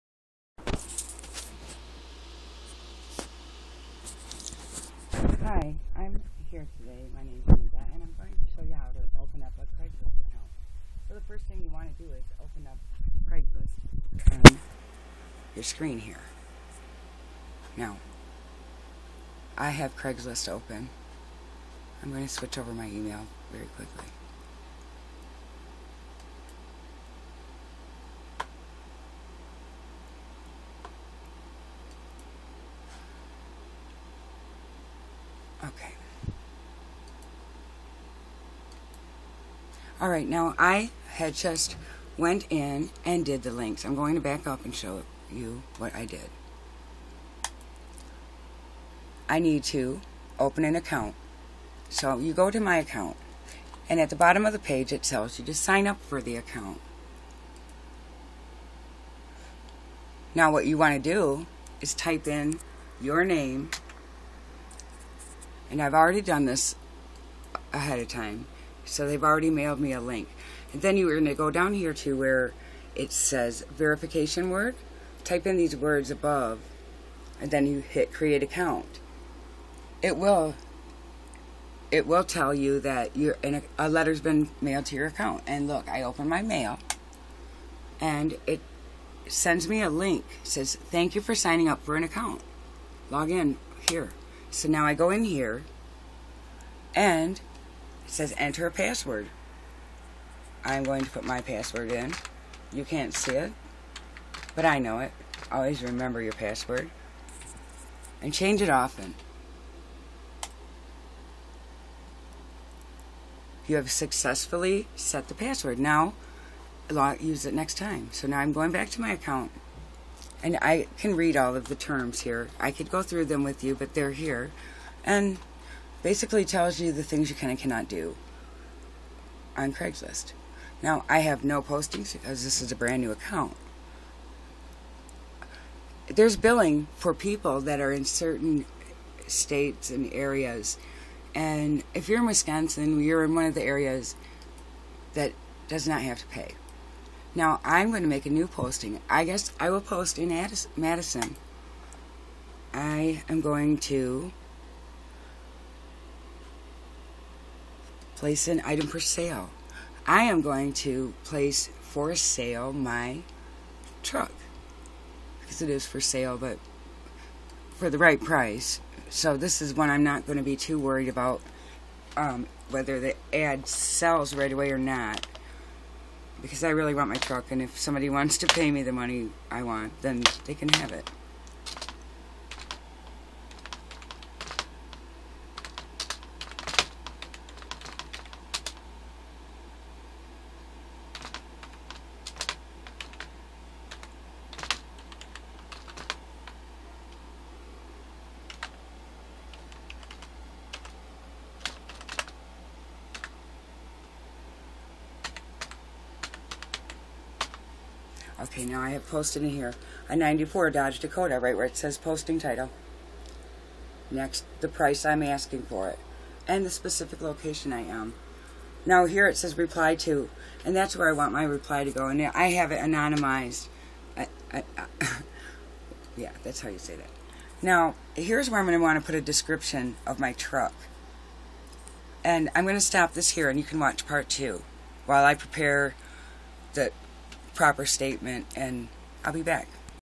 How to post an ad on craigslist for free
This video was uploaded from an Android phone.  How to open an account and sell you car or other things on craigslist for free